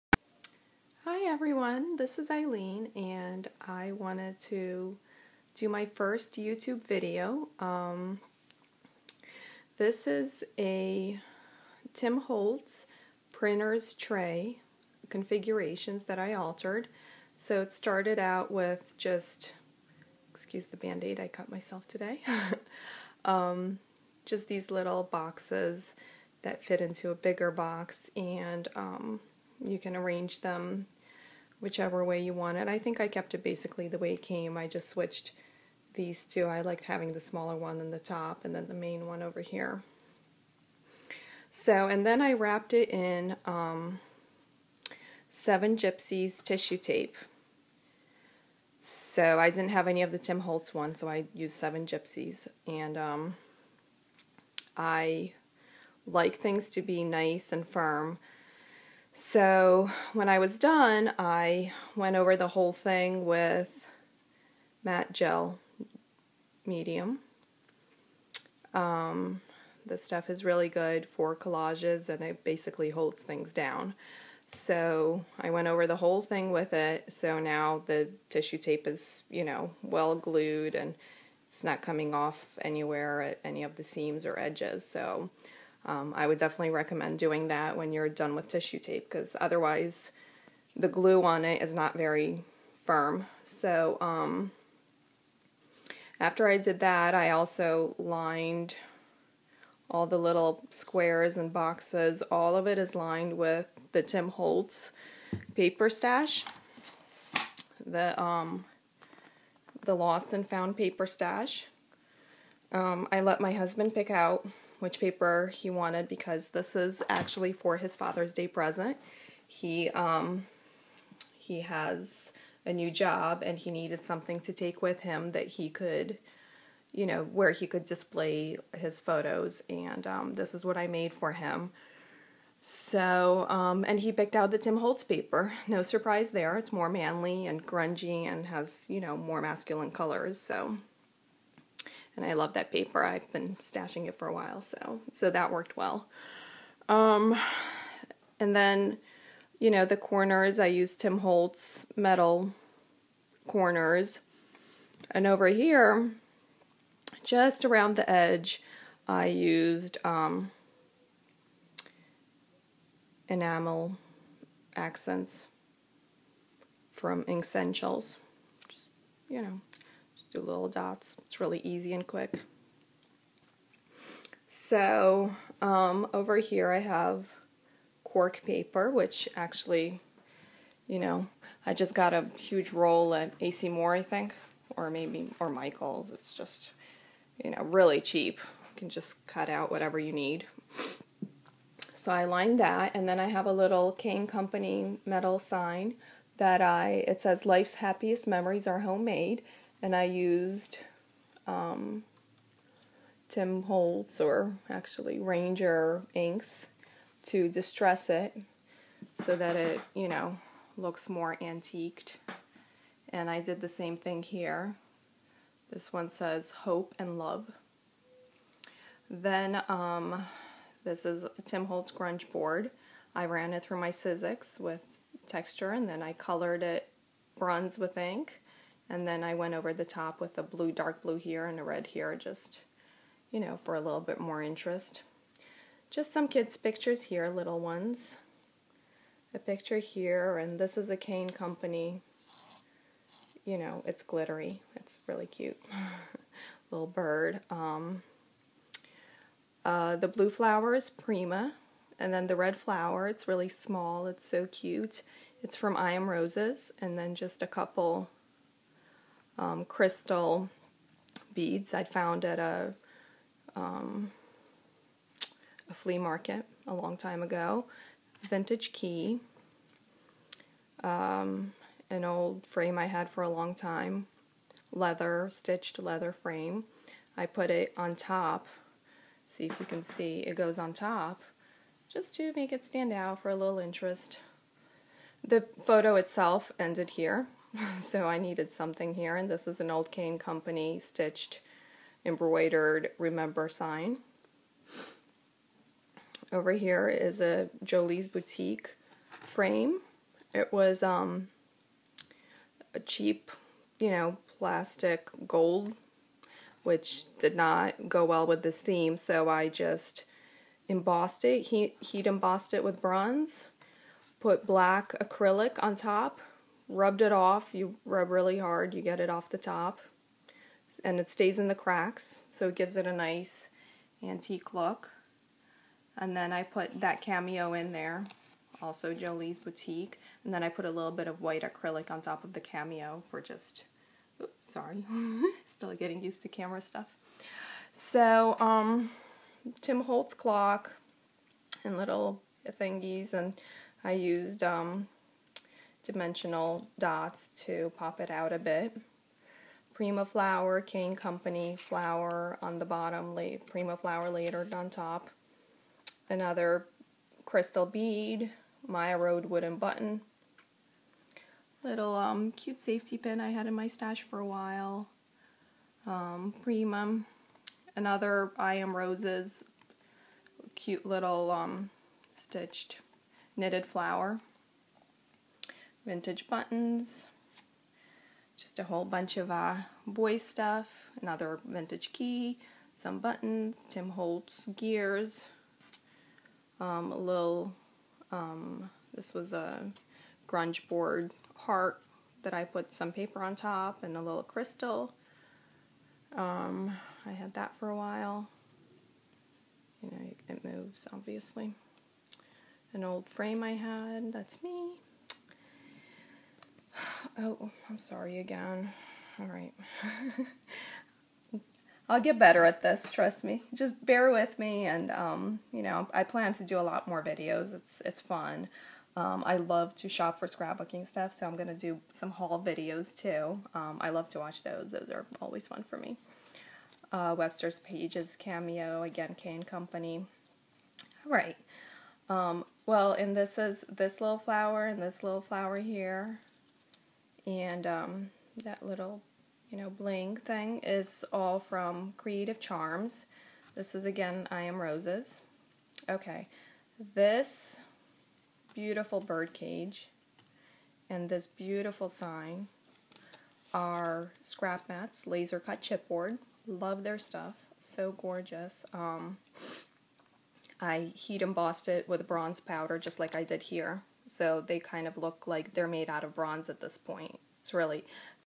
Tim Holtz Configurations Printer's Tray (part 1)
How I altered the configurations printers tray. This is the 1st part - I accidentally hit stop on the camera in the midst of recording and haven't figured out yet how to combine videos. So check out part 2 for the ending, including what I did to the back of the tray.